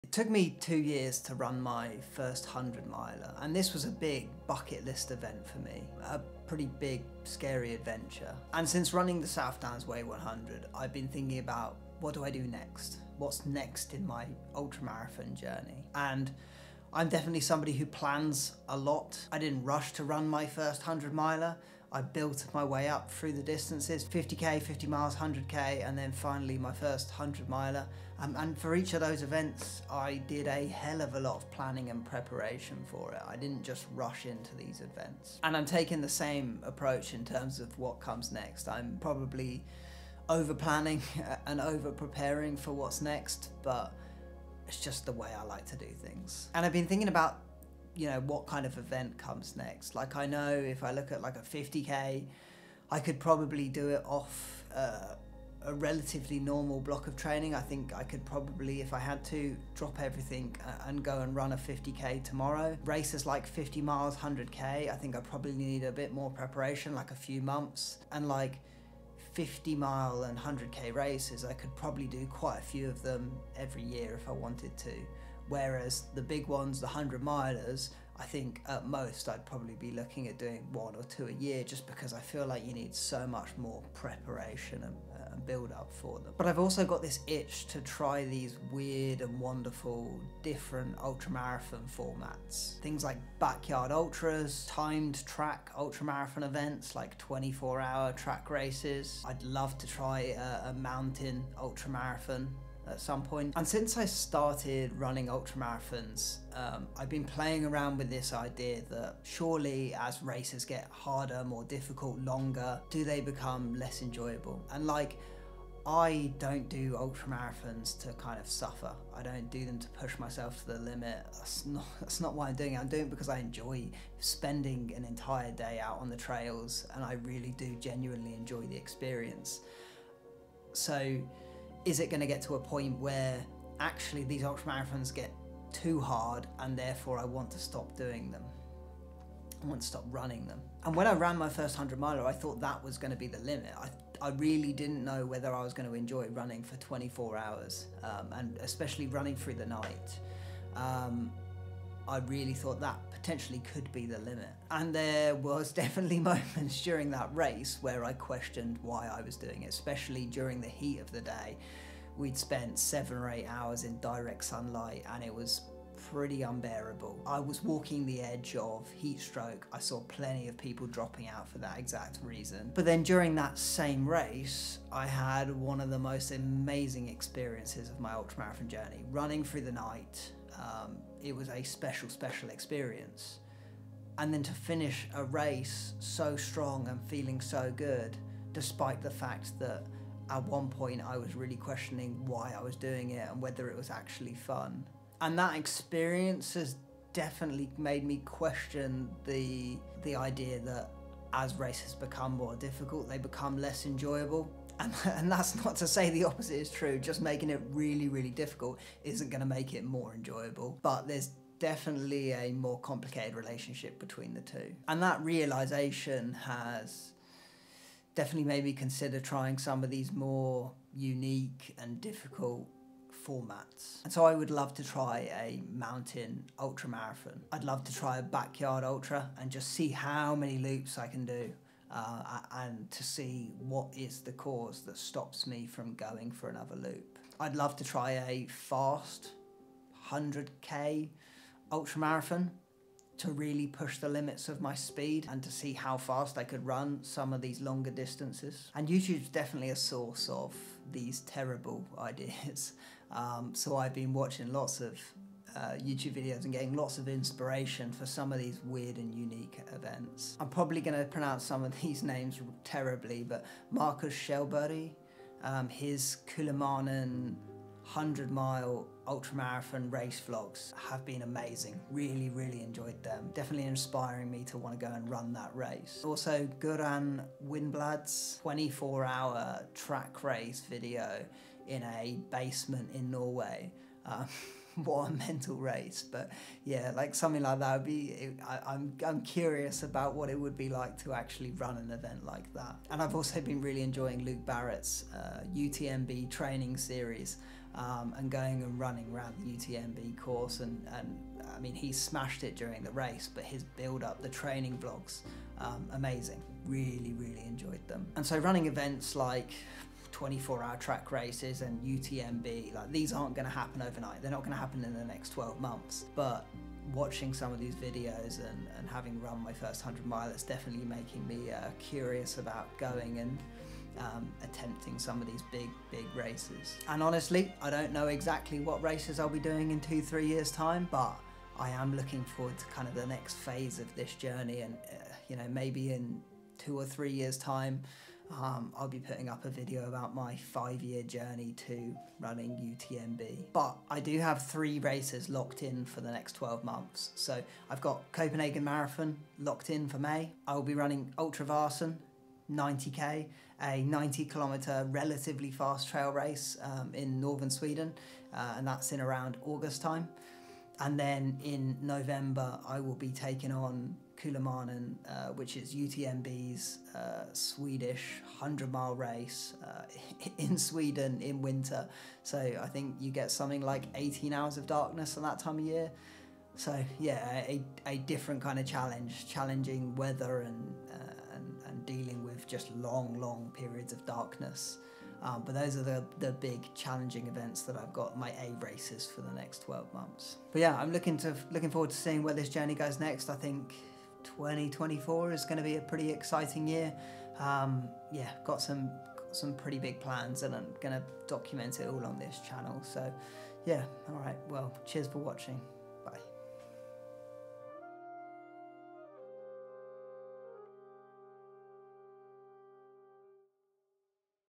What's Next After Running My First 100 Mile Ultramarathon?
In this video, I discuss my future ultramarathon plans. The types of races and the specific ultra marathon events that I plan on running over the next few years.